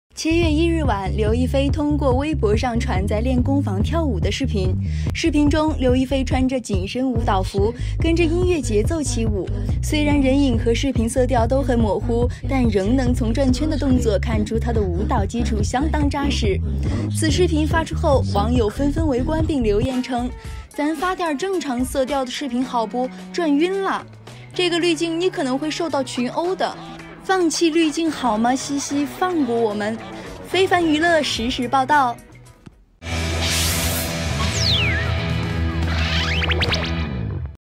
【上個月】劉亦菲曬跳舞視頻看不清人影 網友大呼要求放棄濾鏡！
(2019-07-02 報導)

7月1日晚，劉亦菲通過微博上傳在練功房跳舞的視頻。視頻中，劉亦菲穿著緊身舞蹈服，跟著音樂節奏起舞，雖然人影和視頻色調都很模糊，但仍能從轉圈的動作看出她的舞蹈基礎相當扎實。此視頻曝光后，網友紛紛圍觀并留言稱：“咱發點正常色調的視頻好不？轉暈啦！”“這個濾鏡你可能會受到群毆的。”“放棄濾鏡好嗎茜茜，放過我們。”